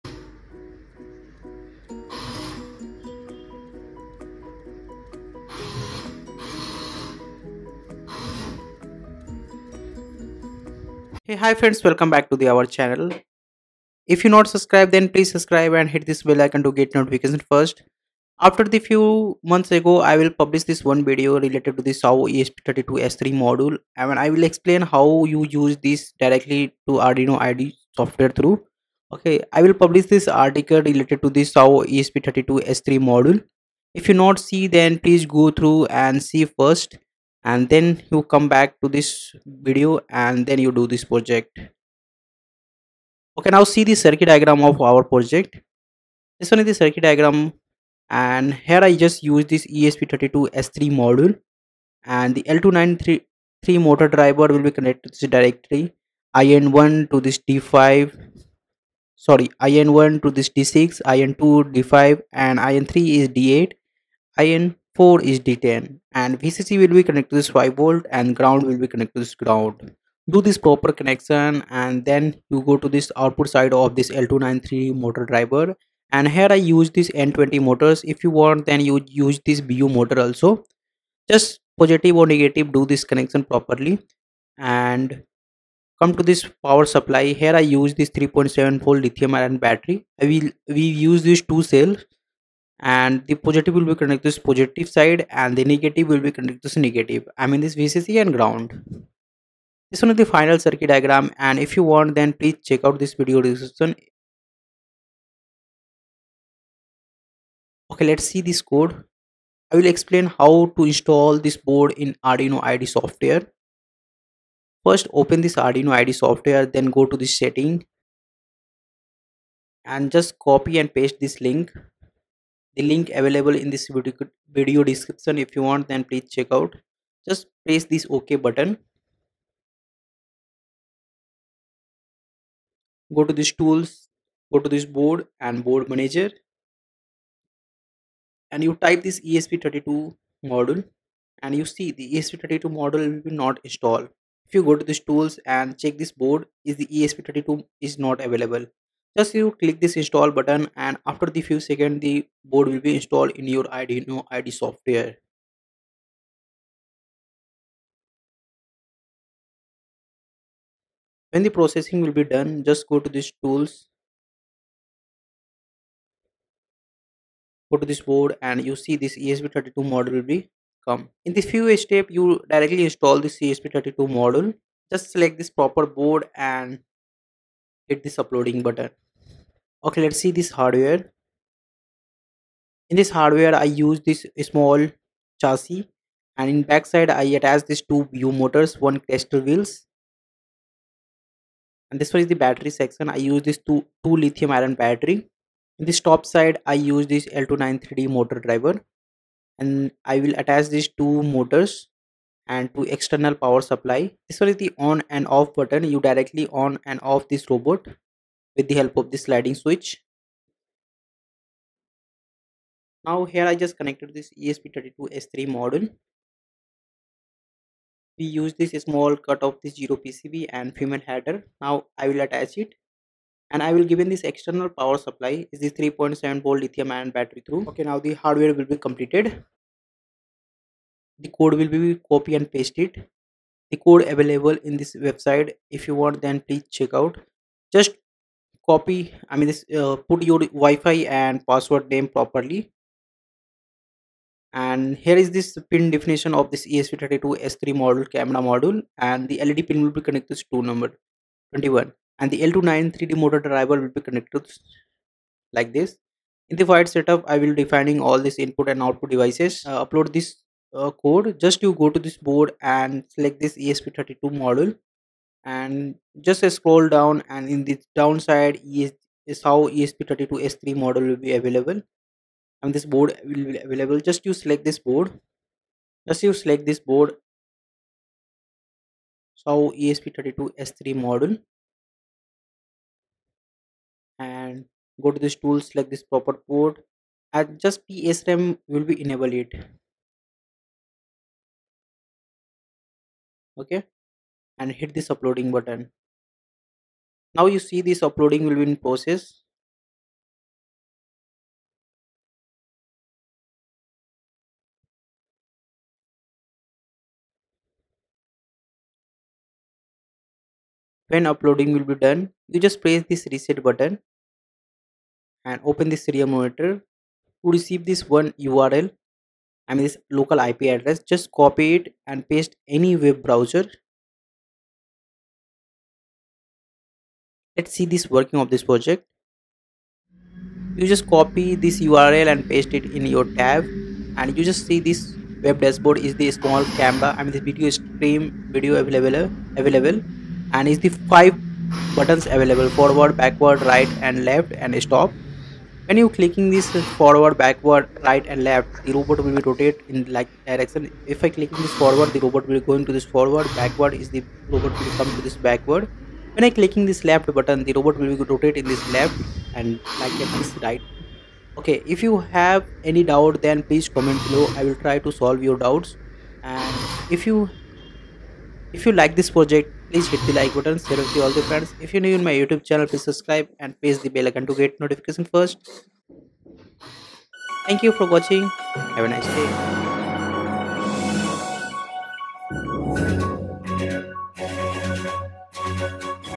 Small Robot Car with Camera Using XIAO ESP32S3 Sense | FPV Robot with Web Control
This project builds a compact robot car equipped with a camera using the XIAO ESP32S3 Sense microcontroller. It features real-time video streaming (FPV) accessible via a local webpage over WiFi, and motor control through an L298N motor driver. The car supports forward, backward, left, right movements and LED lights for camera visibility in low light. The system is programmed via Arduino IDE with user-friendly controls on a web interface.

Component List
Seeed Studio XIAO ESP32S3 Module
L293d motor driver
BO Motor
Wheel
Chassis
3.7v Battery
Zero PCB
Type-C USB Cable